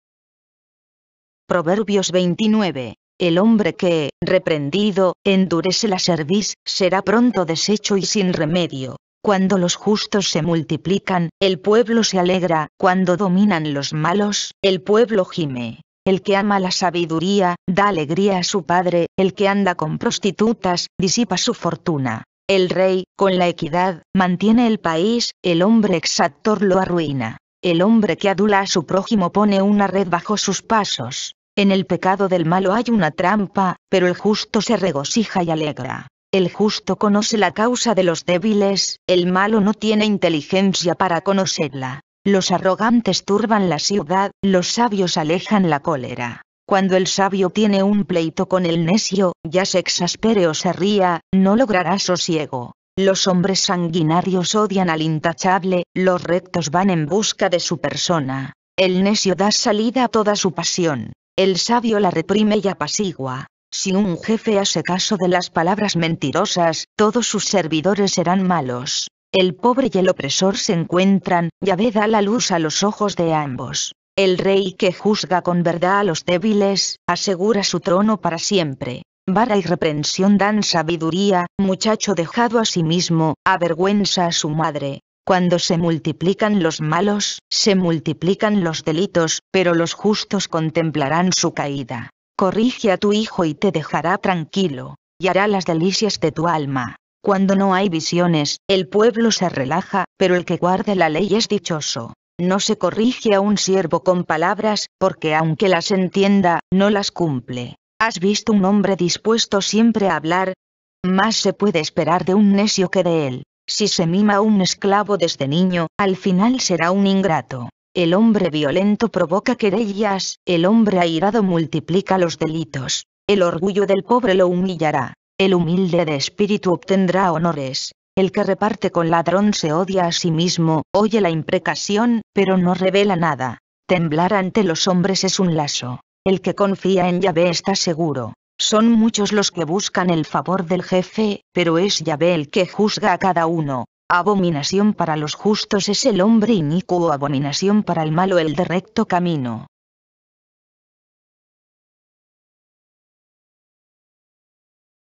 LA SANTA BIBLIA,VERSIÓN BIBLIA DE JERUSALÉN 1976, Proverbios 29
Proverbios 29

 El hombre que, reprendido, endurece la cerviz, será pronto deshecho
y sin remedio.
 Cuando los justos se multiplican, el pueblo se alegra, cuando
dominan los malos, el pueblo gime.
 El que ama la sabiduría, da alegría a su padre, el que anda con
prostitutas, disipa su fortuna.
 El rey, con la equidad, mantiene el país, el hombre exactor lo
arruina.
 El hombre que adula a su prójimo pone una red bajo sus pasos.
 En el pecado del malo hay una trampa, pero el justo se regocija y
alegra.
 El justo conoce la causa de los débiles, el malo no tiene inteligencia
para conocerla.
 Los arrogantes turban la ciudad, los sabios alejan la cólera.
 Cuando el sabio tiene un pleito con el necio, ya se exaspere o se ría,
no logrará sosiego.
 Los hombres sanguinarios odian al intachable, los rectos van en
busca de su persona.
 El necio da salida a toda su pasión; el sabio la reprime y apacigua.
 Si un jefe hace caso de las palabras mentirosas, todos sus
servidores serán malos.
 El pobre y el opresor se encuentran, Yahveh da la luz a los ojos de
ambos.
 El rey que juzga con verdad a los débiles, asegura su trono para
siempre.
 Vara y reprensión dan sabiduría, muchacho dejado a sí mismo,
avergüenza a su madre.
 Cuando se multiplican los malos, se multiplican los delitos, pero
los justos contemplarán su caída.
 Corrige a tu hijo y te dejará tranquilo; y hará las delicias de tu
alma.
 Cuando no hay visiones, el pueblo se relaja, pero el que guarda la
ley es dichoso.
 No se corrige a un siervo con palabras, porque aunque las entienda,
no las cumple.
 ¿Has visto un hombre dispuesto siempre a hablar? más se puede
esperar de un necio que de él.
 Si se mima a un esclavo desde niño, al final será un ingrato.
 El hombre violento provoca querellas, el hombre airado multiplica
los delitos.
 El orgullo del pobre lo humillará; el humilde de espíritu obtendrá
honores.
 El que reparte con ladrón se odia a sí mismo, oye la imprecación,
pero no revela nada.
 Temblar ante los hombres es un lazo; el que confía en Yahveh está
seguro.
 Son muchos los que buscan el favor del jefe, pero es Yahveh el que
juzga a cada uno.
 Abominación para los justos es el hombre inicuo abominación para
el malo el de recto camino.